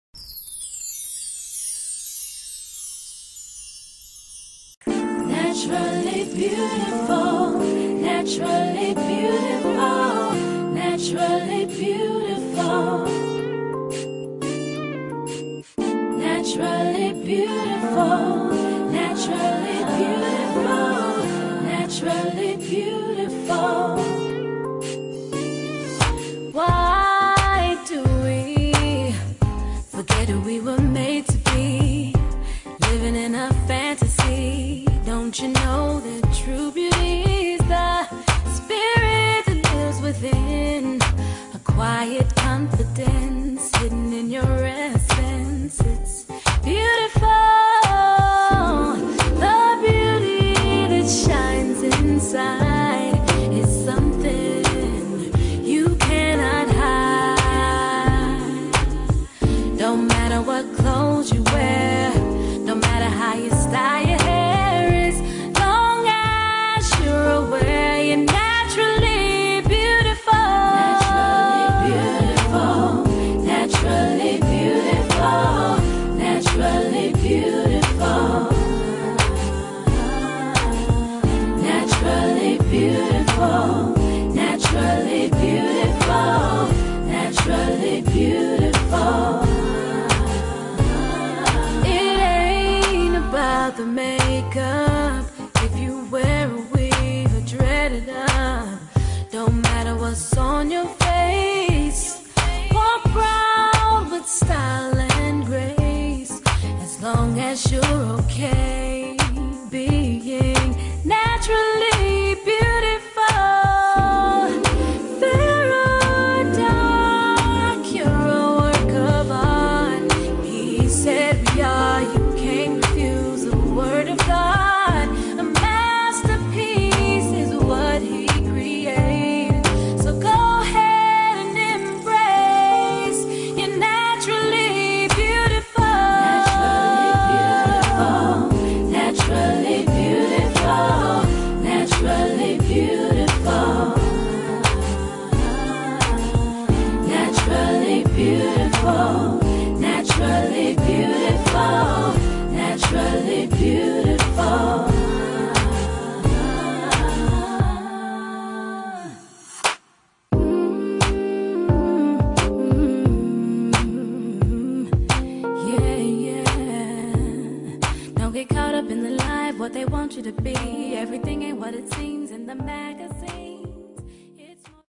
LuvAlwayzEndurez IS BACK!!!
I just passed my 1 year FULLY natural anniversary (nappiversary) on December 19, 2011. Please celebrate with me! I am looking forward to posting many new videos in the upcoming year. Questions? Comments? Requests? Please, let me know!